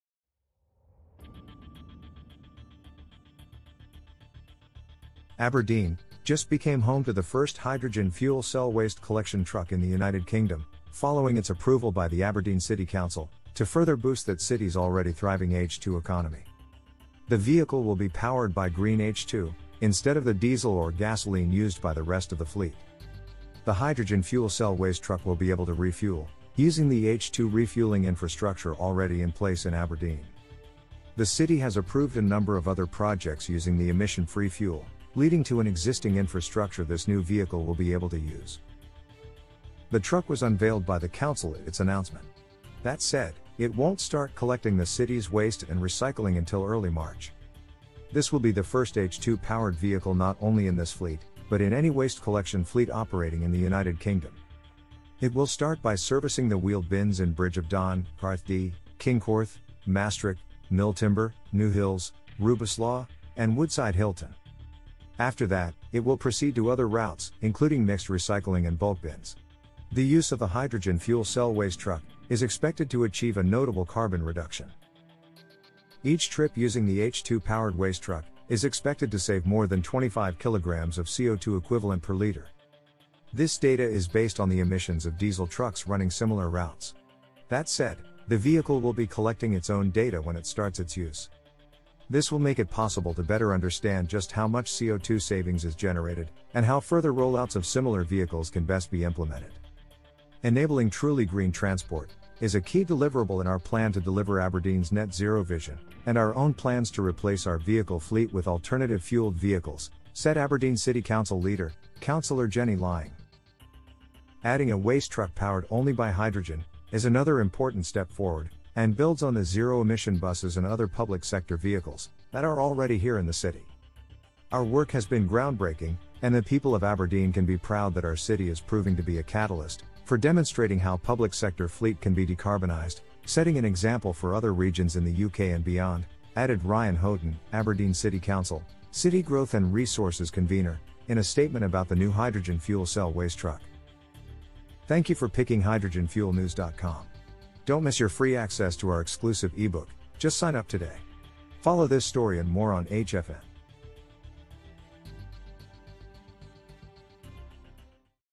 First hydrogen fuel cell waste truck in the UK to roll out on Aberdeen streets
The City Council has given the green light to use the truck to boost the city’s H2 economy...